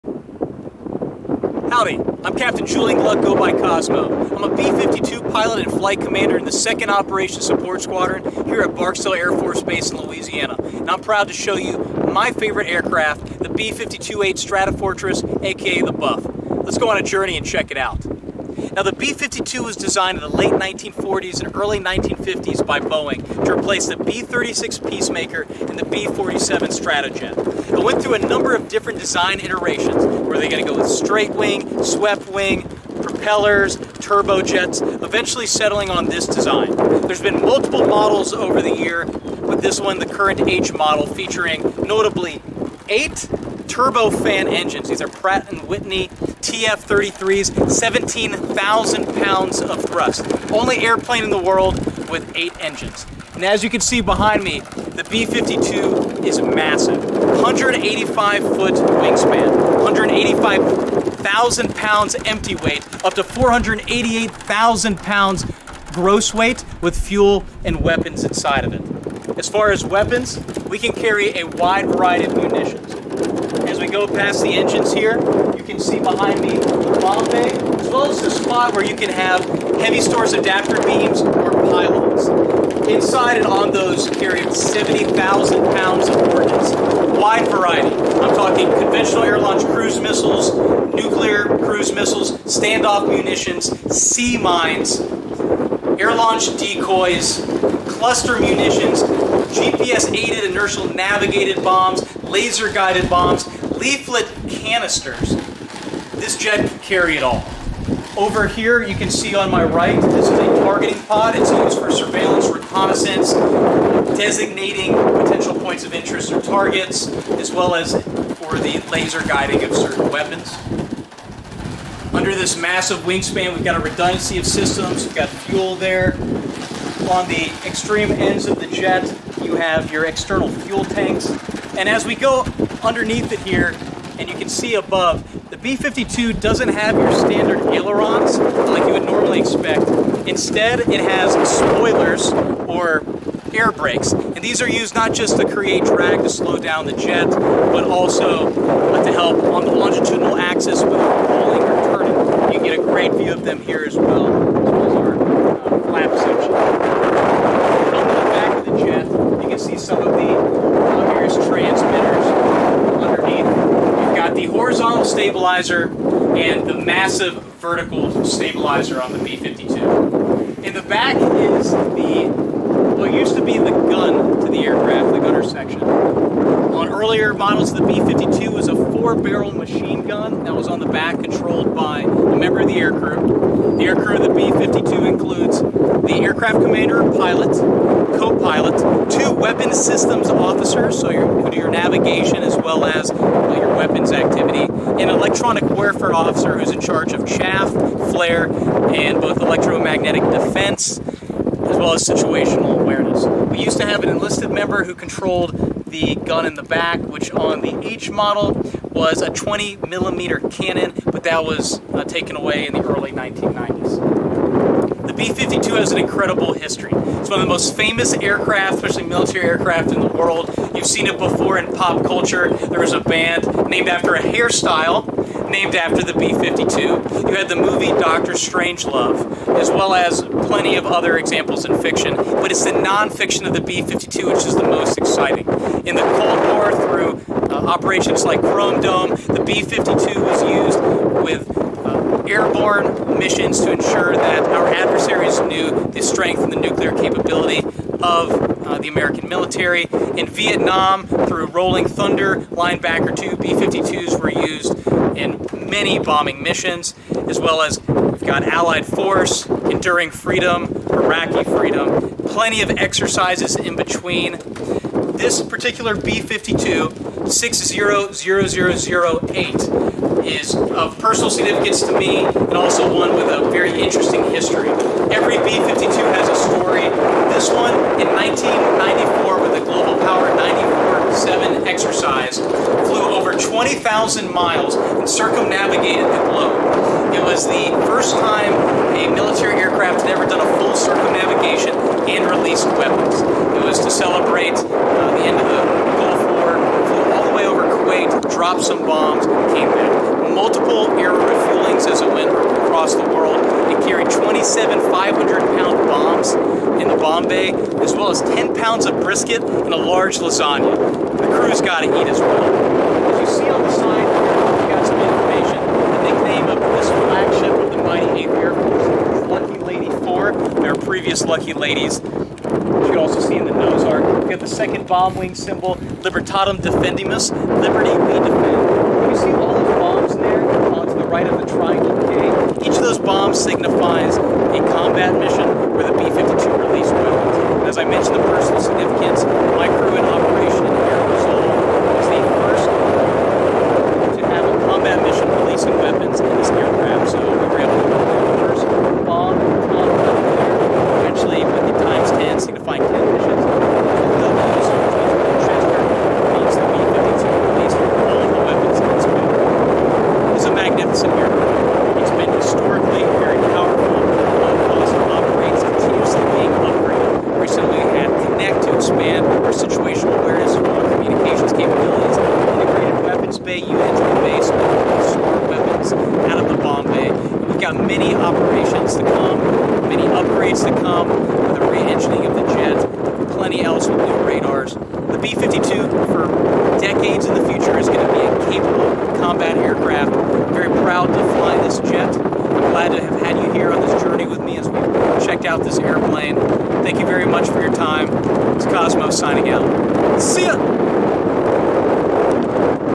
Capt. Julian Gluck, 2012 grad, gives a tour of the B-52
An inside look at the B-52 from Barksdale, AFB!